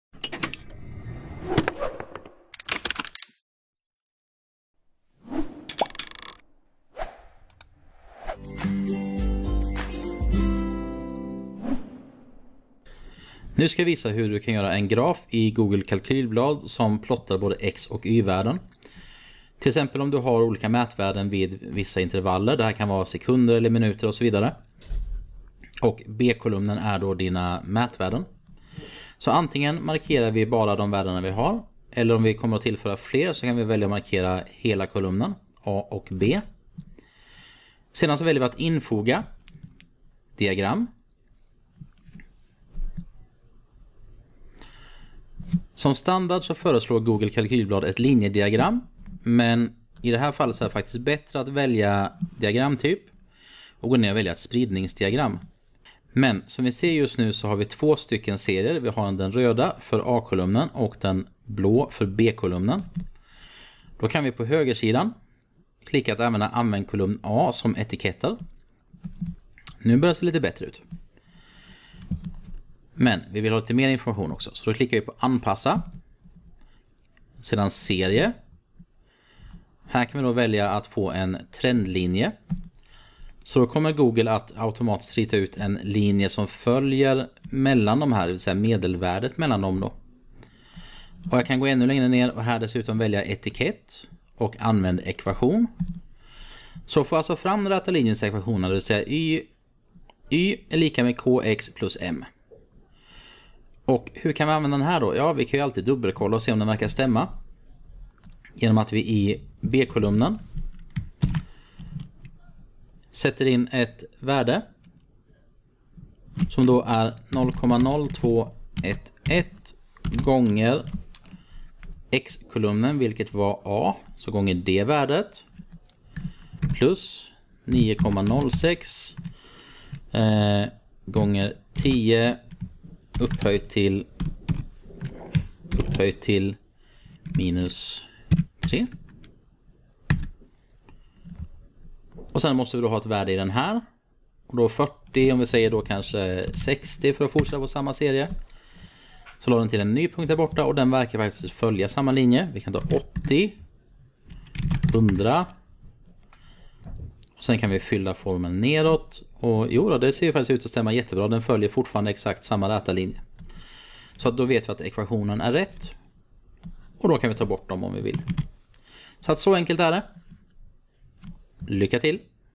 Graf med X och Y värden i Google Kalkylblad
En snabb introduktion hur du får Google kalkylblad att göra ett diagram där du själv anger både x och y värde. Dessutom trendlinje och ekvation för linjen.